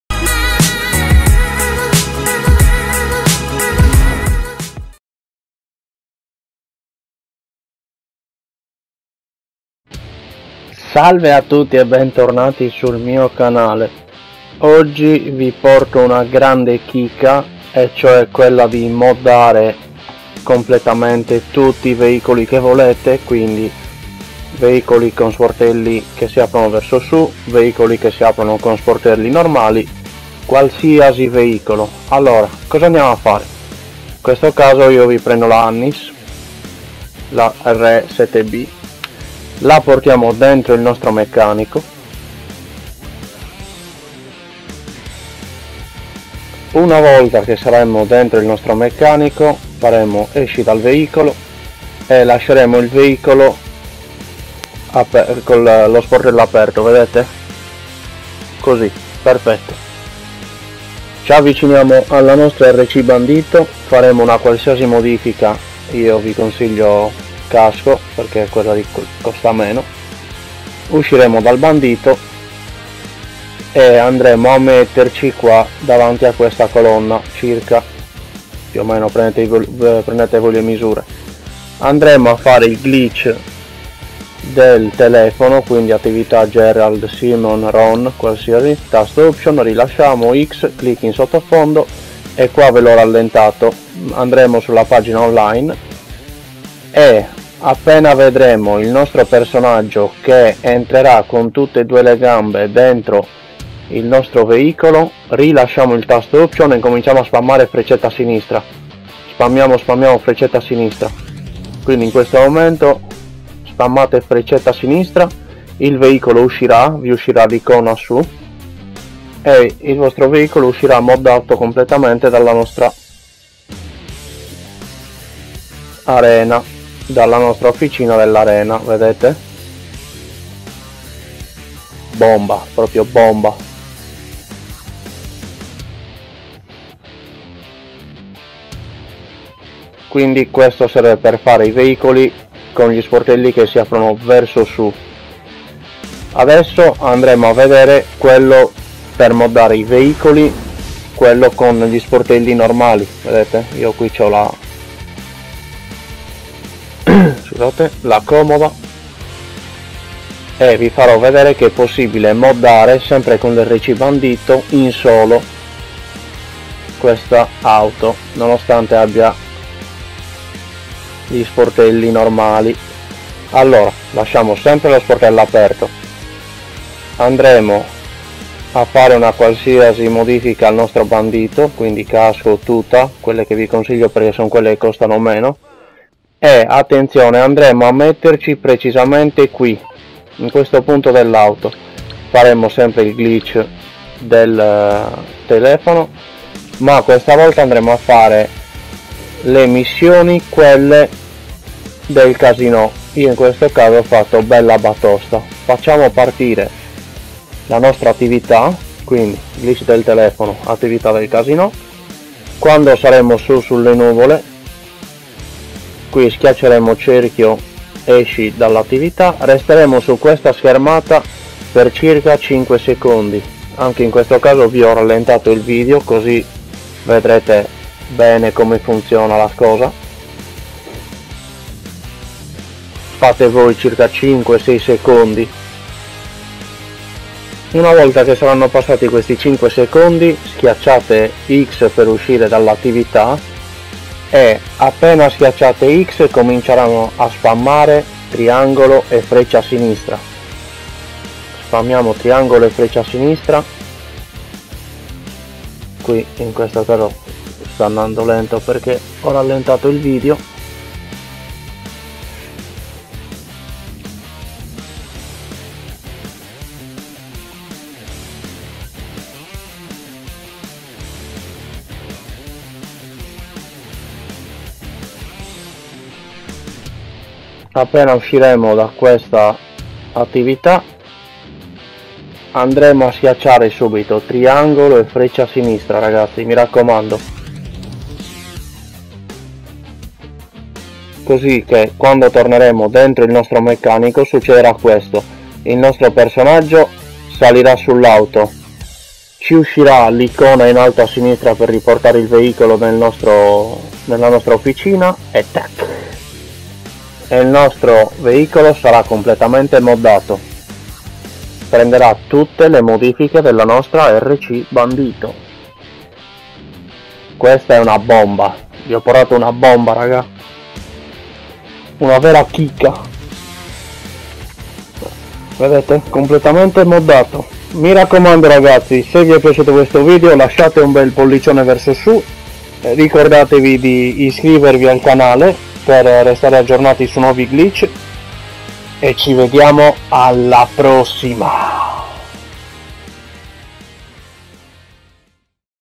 *PATCHATO* 💣VIDEO BOMBA💣 ⚠️ *MODDARE QUALUNQUE VEICOLO CON L’RC TO CAR*⚠️
💣💣💣Oggi pezzo bomba💣💣💣
Salve a tutti ragazzi 😎 oggi vi porto un nuovo metodo per moddare tutti i vostri veicoli con sportellò che aprono verso su o normalmente usando sempre L’RC to CAR!

⚠️RC TO CAR⚠️